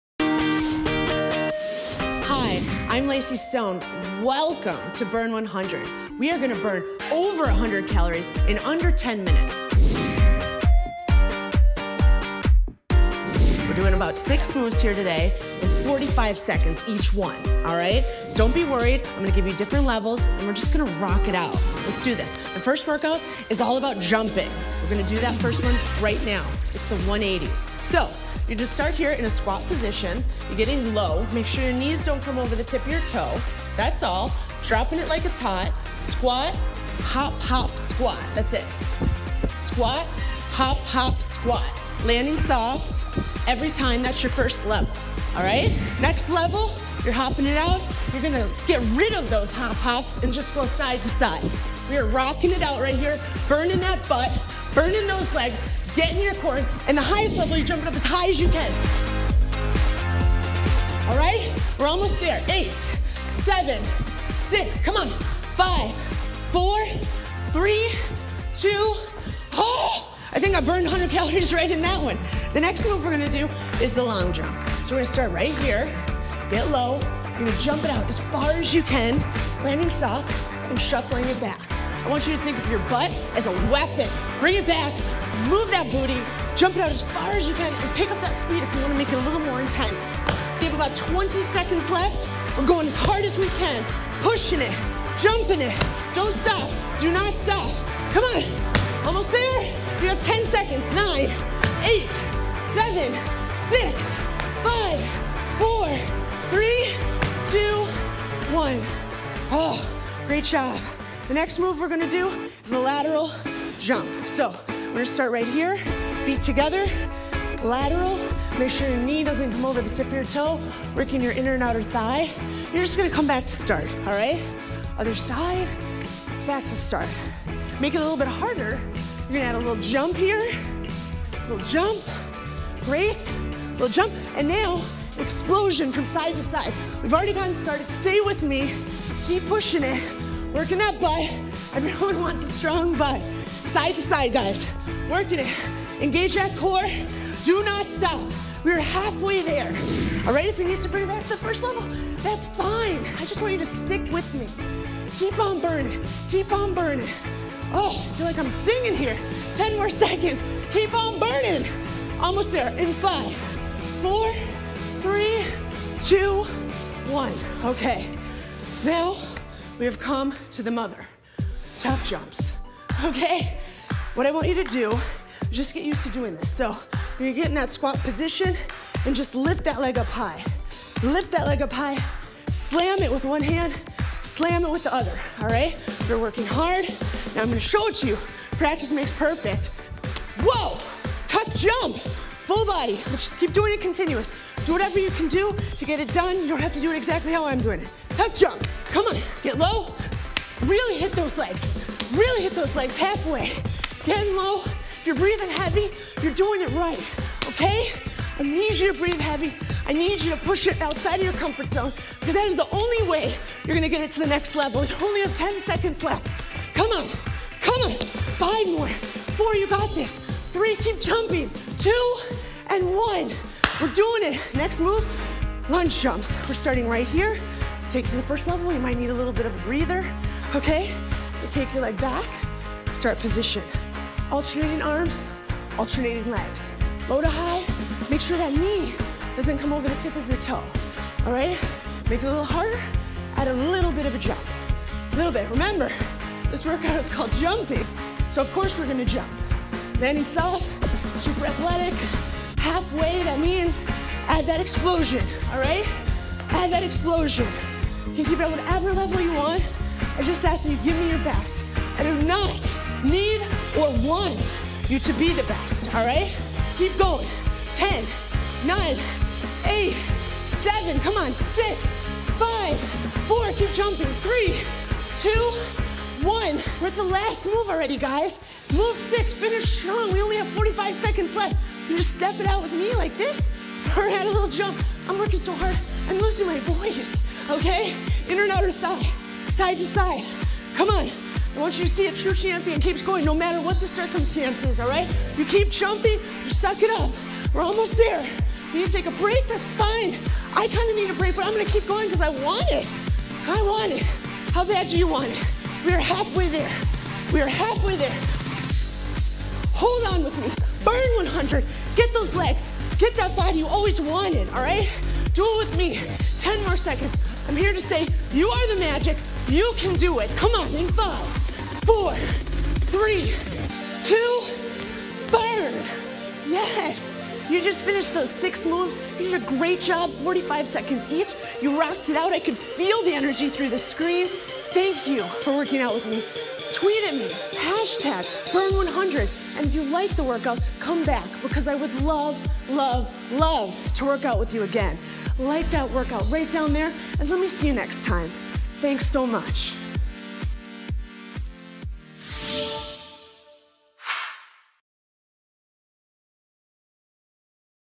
想瘦就跳起來！6個超簡單「跳躍運動」教學｜ 健身教室｜Vogue Taiwan
跳躍雖然是很簡單的動作，其實它有很多種變化，
除了是一個全身性的運動，卻能增加骨頭密度、骨頭的彈性與韌性。